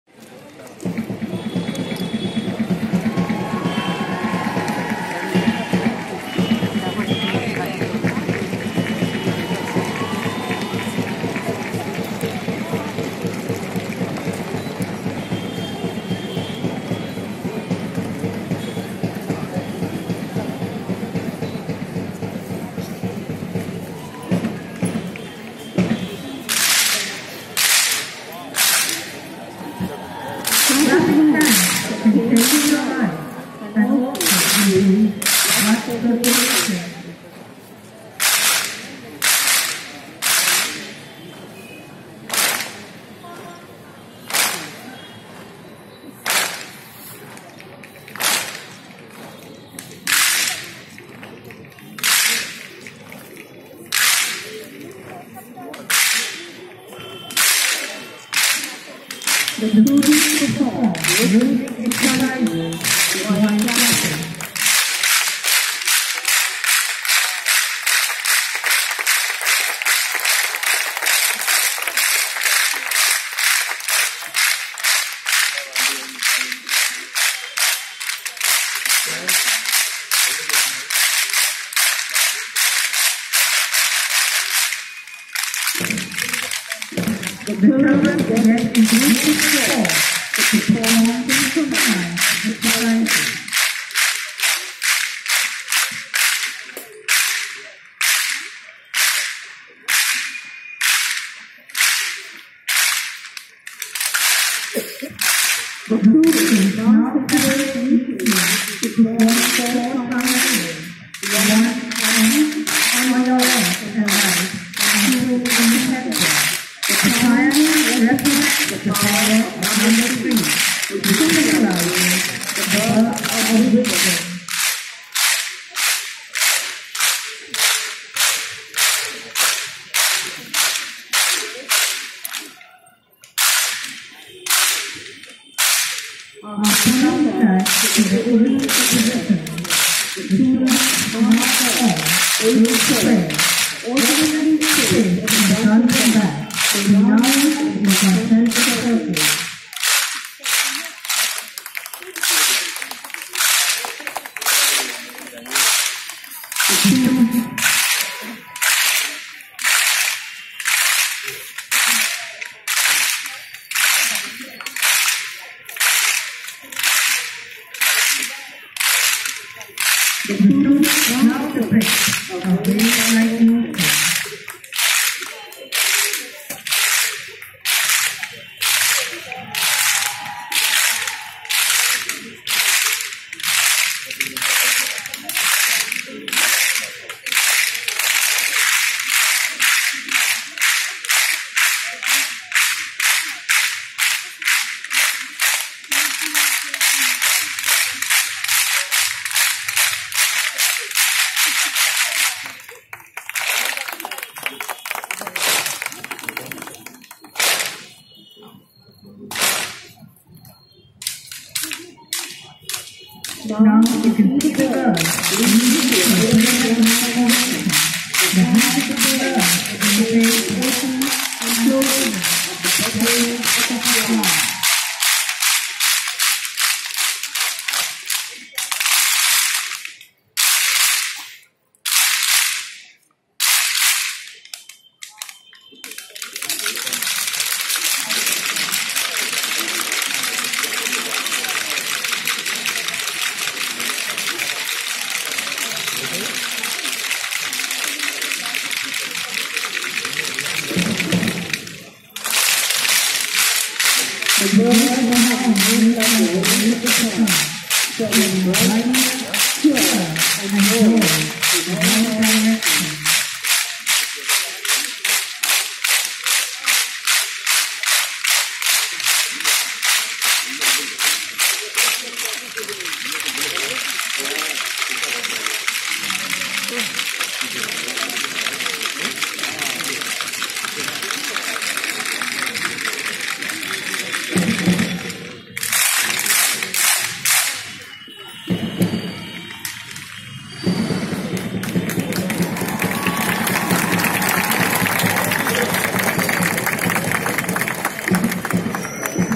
Clapping Drill by Class 9 LMGC 2017
In this video, the students will show you how clapping is good and healthy for your health!